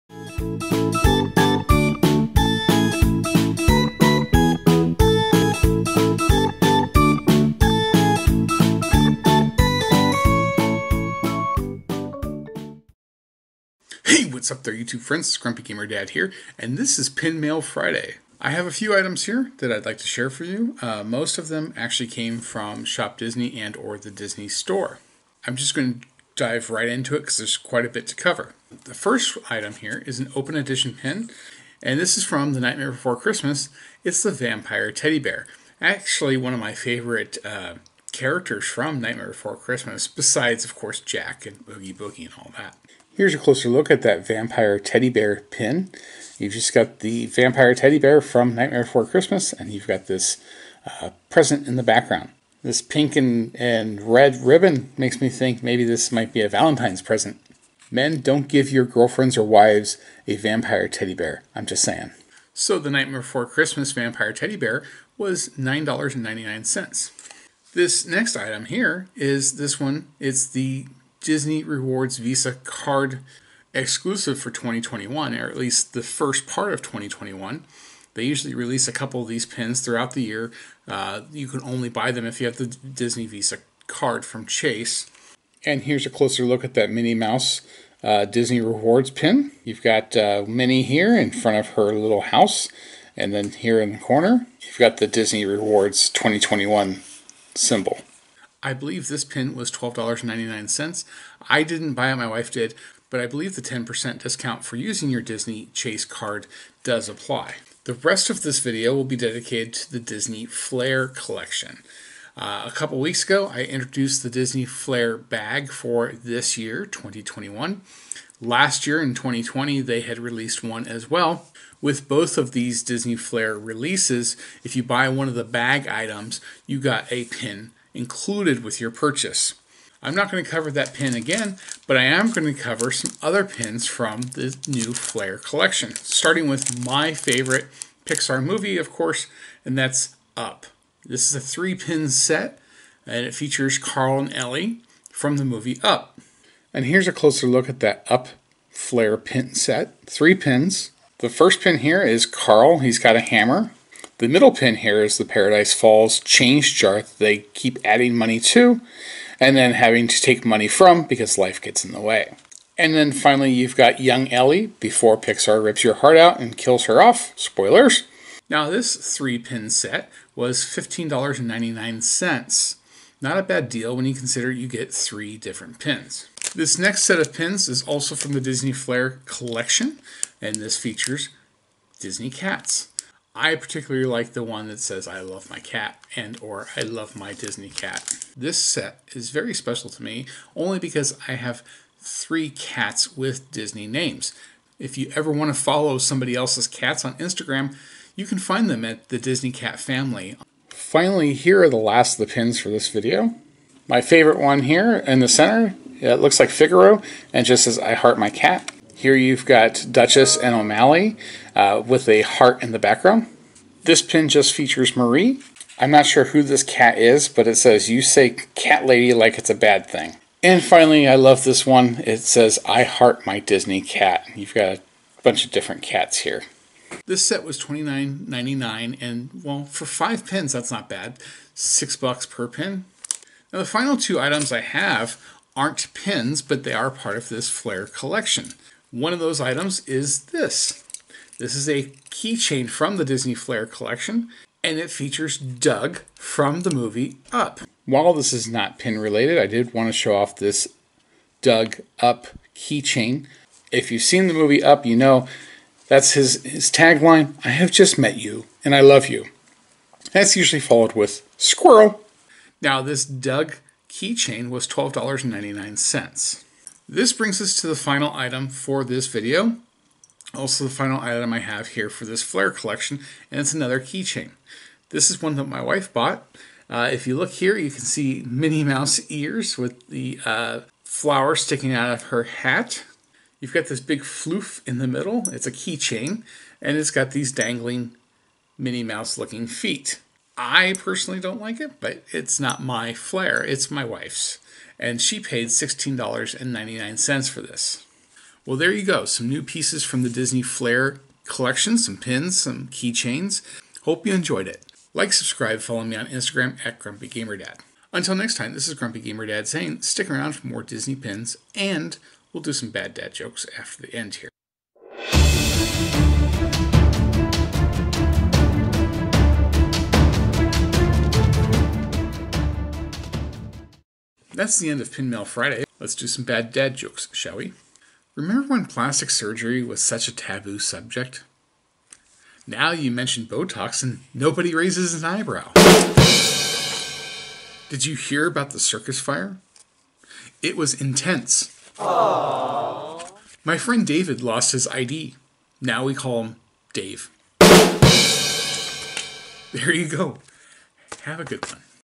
New Disney Flair Pins
It's Pin Mail Friday and in this video, we check out some new open edition pins including the new Disney Rewards pin and new items from the Disney Flair collection.